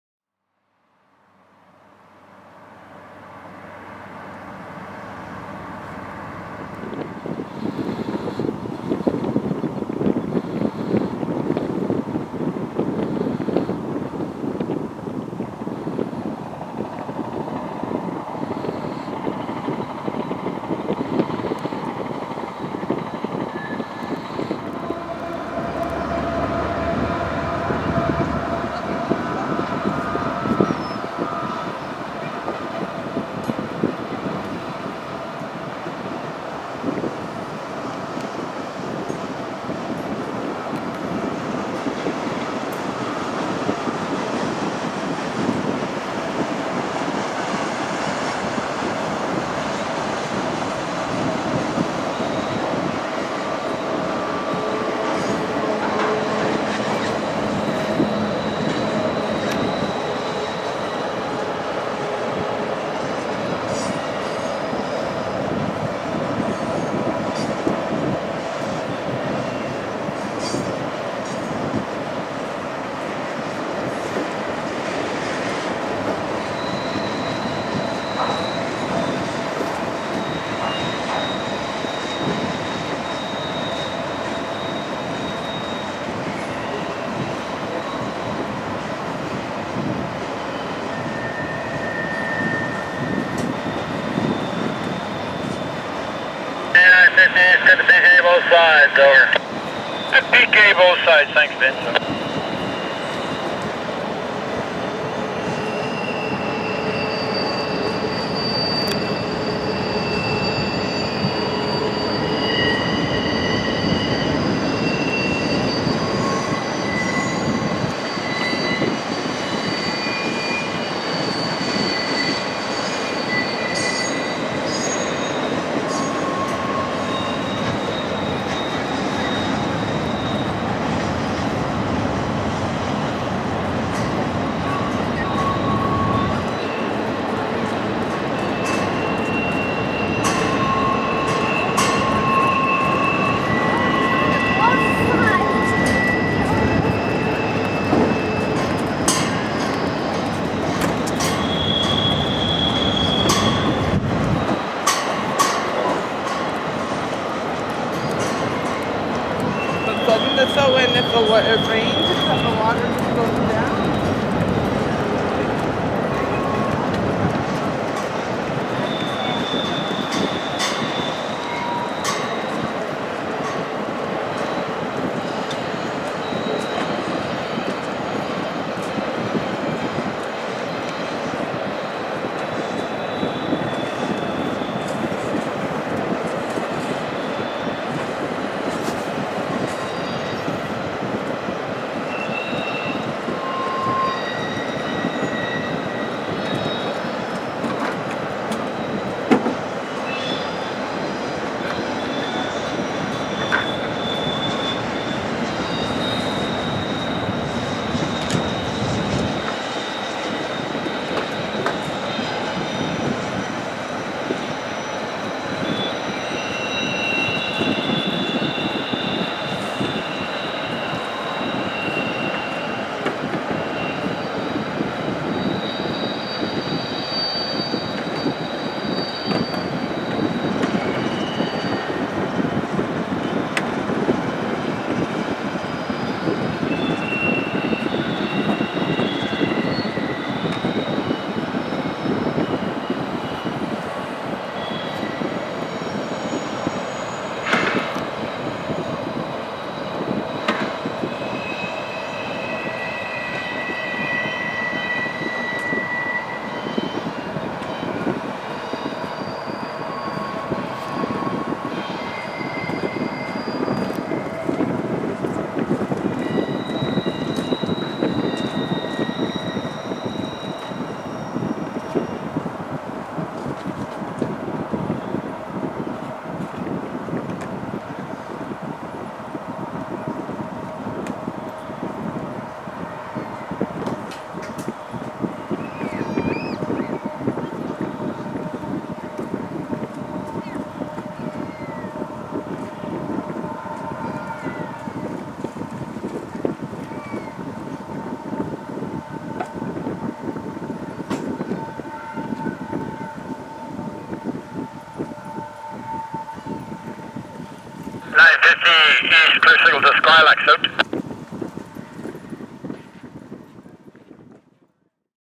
CP 8950 at Chase (24AUG2013)
Canadian Pacific 8950, CP 8723 (Train 100 -- Vancouver to Montreal) (450 axles)
Shuswap Subdivision
Chase, British Columbia